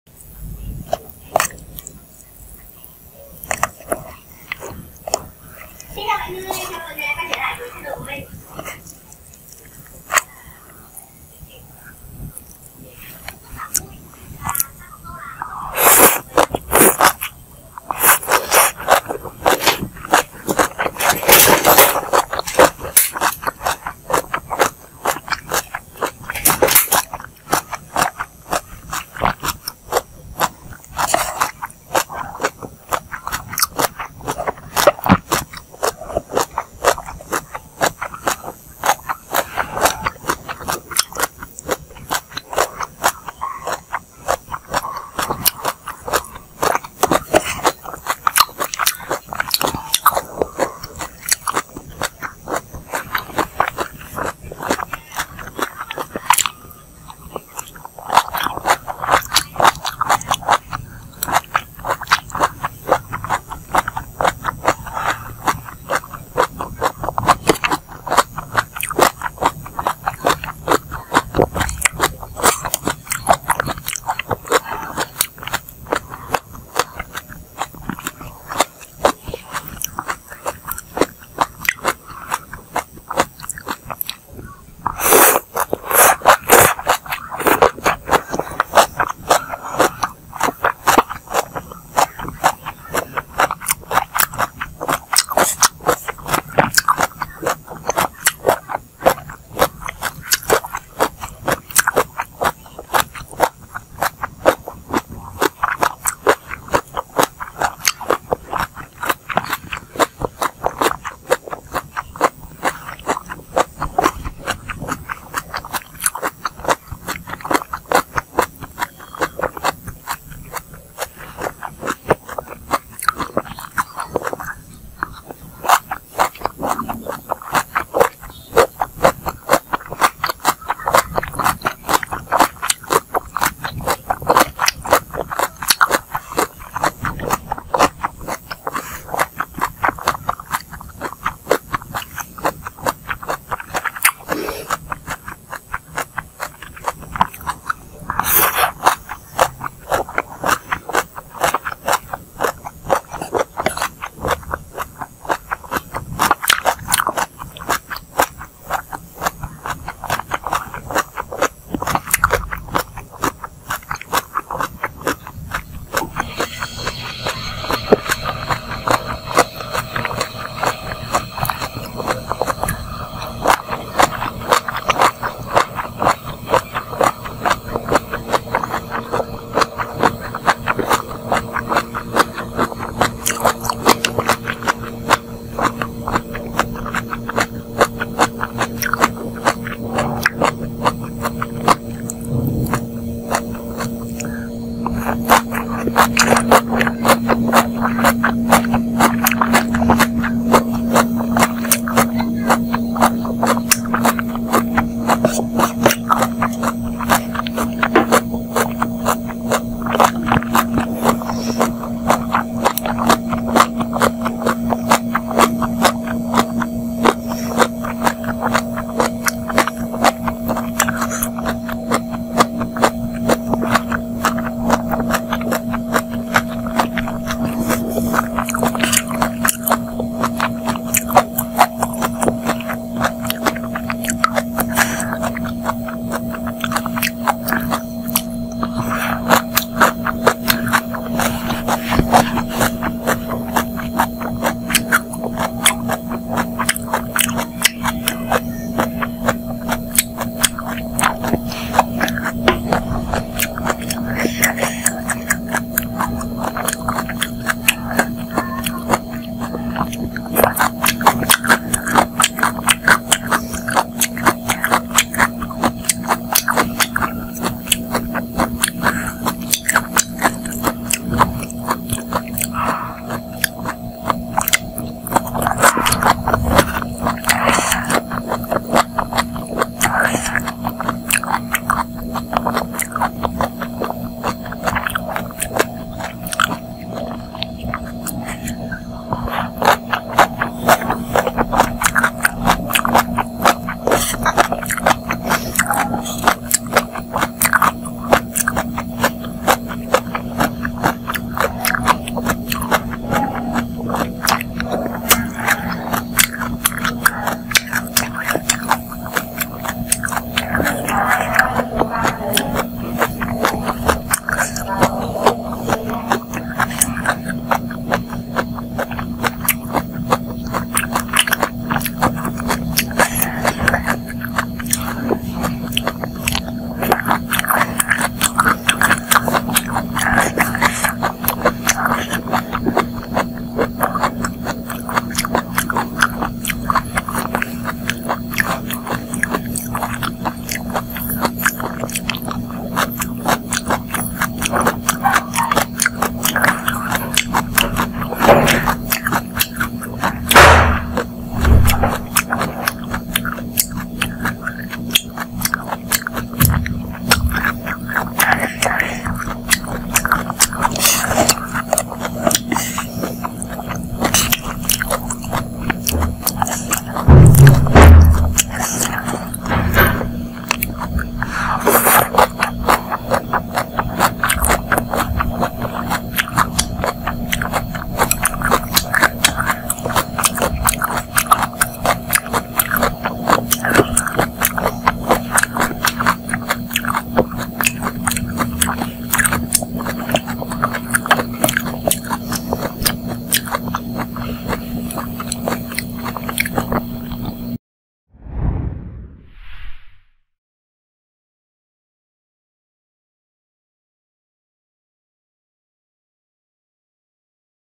ASMR RAW JELLYFISH WITH SALAD (EXOTIC FOOD) EXTREME CRUNCHY eating sound
Jellyfish mixed with lemon juice, carrots, kohlrabi, roasted peanuts, marjoram, garlic, sugar, fresh peppers help the dish become attractive. Each piece of crispy fresh jellyfish flavored with the sea blends with each slice of crispy vegetables and fruits. Fresh and spicy dishes with sweet and salty taste mingle with each other.